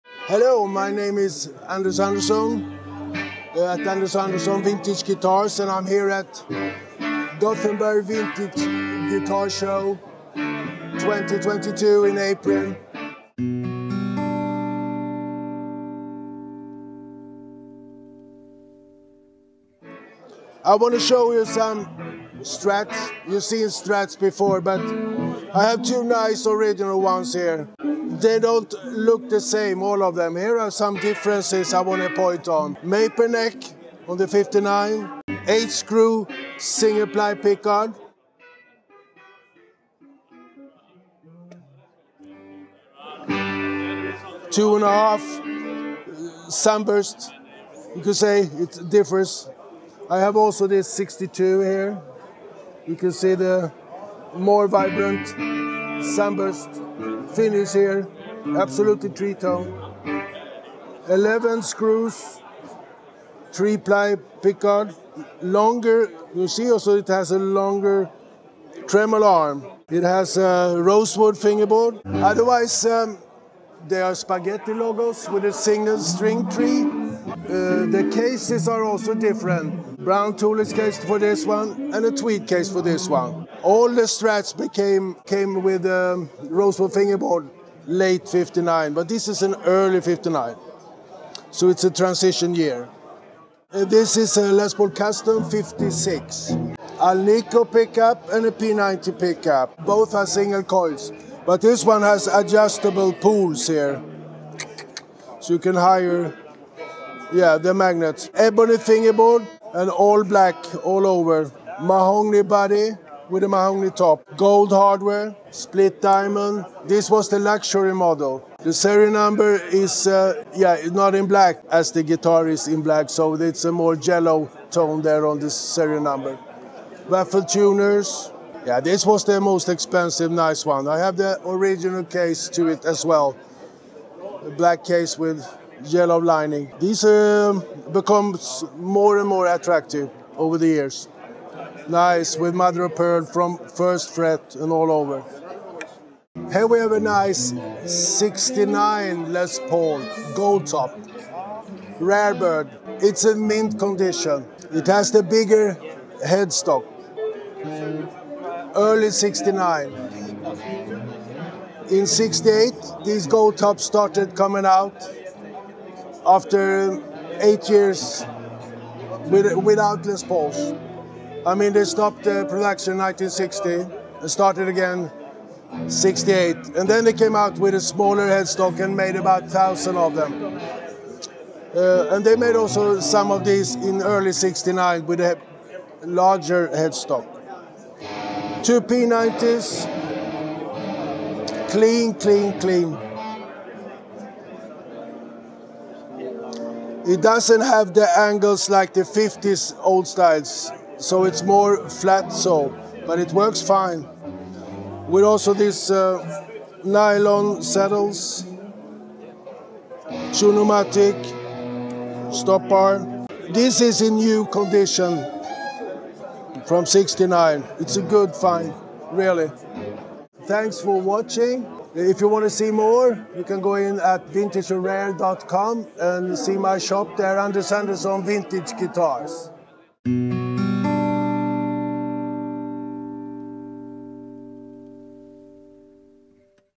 Rare & Collectible Vintage Guitars | Gothenburg Vintage Guitar Show | Anders Anderson
We sat down with Swedish vintage guitar dealer Anders Anderson at Gothenburg Vintage Guitar Show 2022. In this video, Anders presents 4 amazing vintage guitars: a 1959 Fender Stratocaster, a 1962 Fender Stratocaster, a 1956 Gibson Les Paul Custom, and a 1969 Gibson Les Paul Goldtop.
Shop the world´s finest instruments directly from dealers, builders, and private sellers